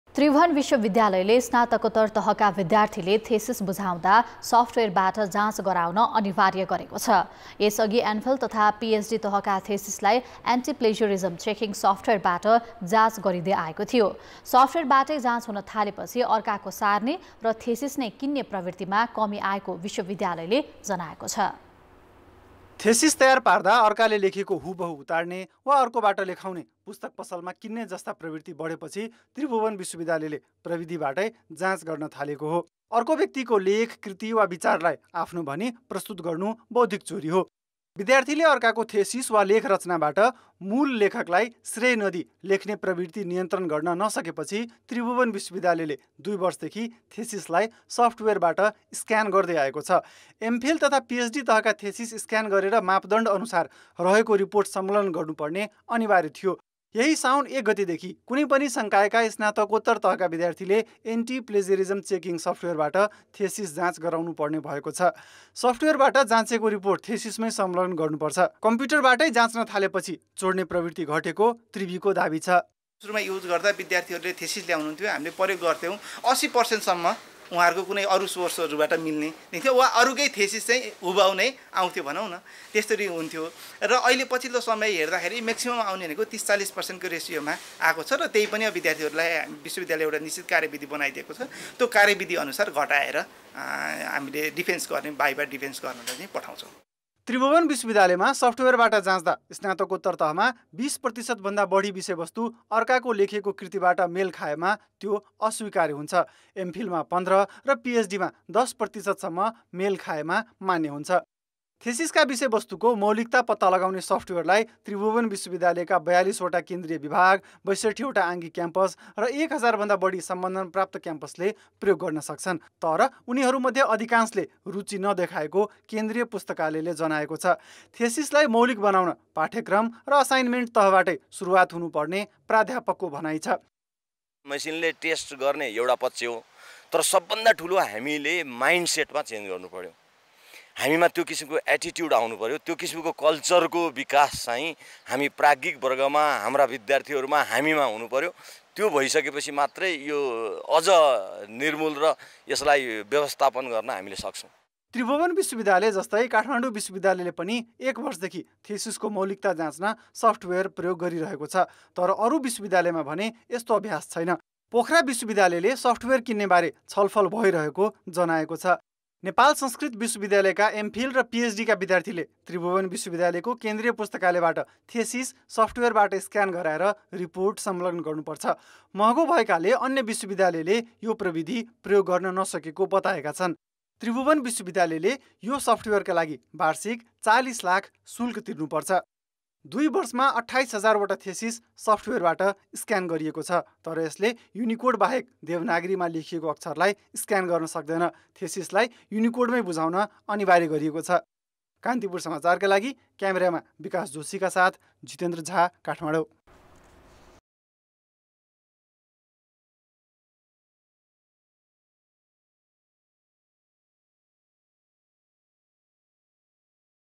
स्नातकोत्तर तहका विद्यार्थीको थेसिस अबदेखि सफ्टवेयरबाट जाँच गरिने | Kantipur Samachar
त्रिभुवन विश्वविद्यालयले स्नातकोत्तर तहका विद्यार्थीले थेसिस बुझाउँदा सफ्टवेयरबाट जाँच गराउन अनिवार्य गरेको छ । यसअघि एमफिल तथा पीएचडी तहका थेसिसलाई एन्टी–प्लेजरिजम चेकिङ सफ्टवेयरबाट जाँच गरिँदै आएको थियो । सफ्टवेयरबाटै जाँच हुन थालेपछि अर्काको सार्ने र थेसिस नै किन्ने प्रवृत्तिमा कमी आएको विश्वविद्यालयले जनाएको छ ।